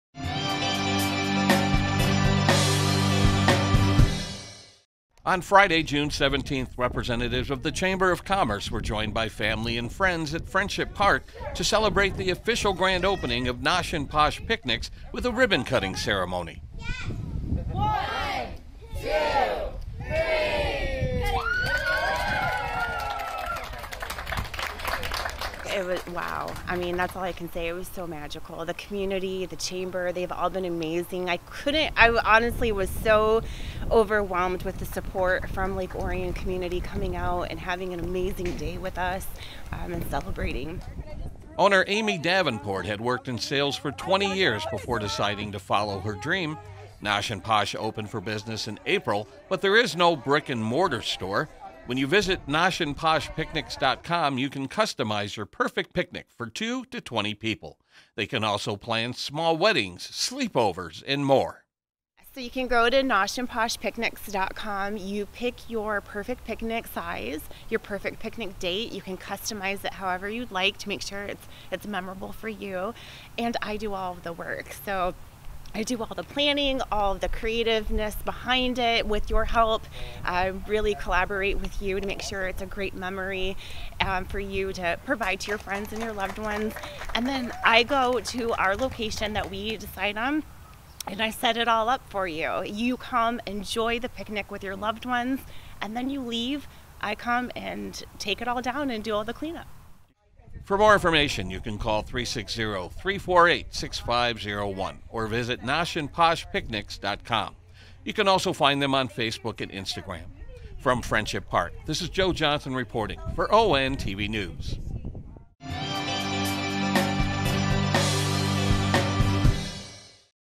ONTV News Break: Nosh N Posh Picnics
On Friday, June 17, 2022, the Orion Area Chamber helped celebrate the official grand opening of Nosh N Posh Picnics with a ribbon-cutting ceremony at Friendship Park.